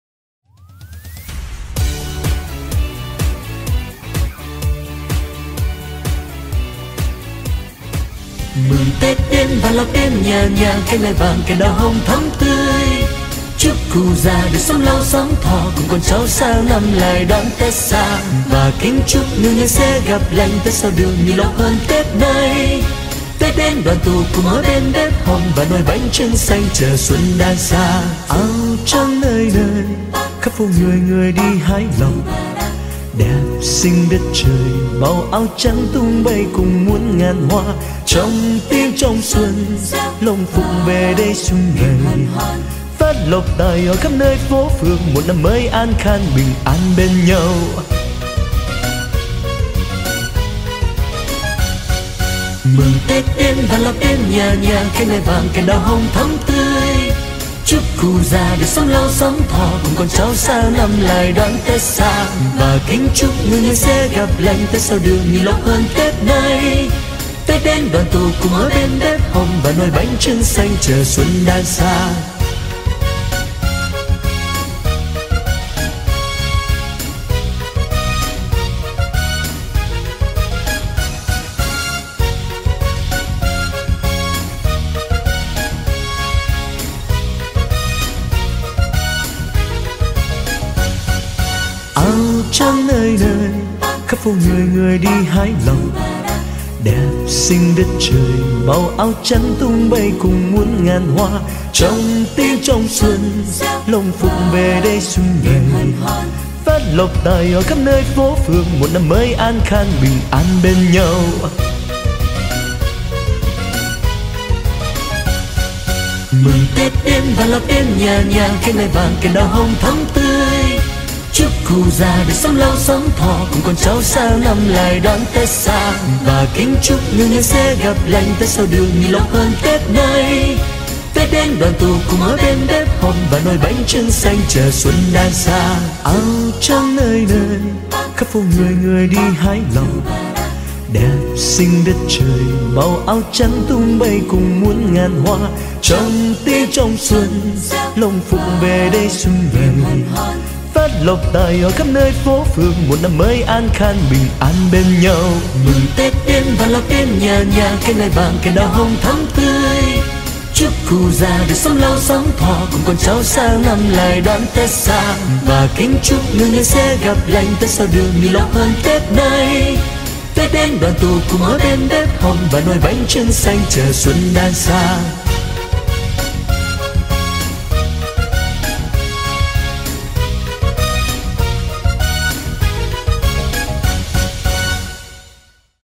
Asiana Travel - Happy Vietnamese New Year 2018
Asiana Travel Team wish you Healthy & Wealthy.
On behalf of 54 Ethnic Minorities in Vietnam, We would like to sharing an Atmosphere of Vietnam Lunar New Year 2018